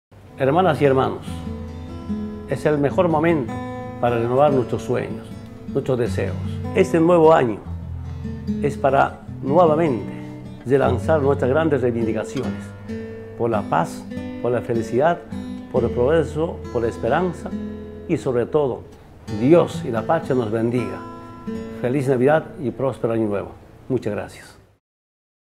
MENSAJE PRESIDENTE EVO MORALES 2015
Por la paz, por la felicidad, por el progreso, la esperanza, Dios y la Patria les bendiga, Felices Fiestas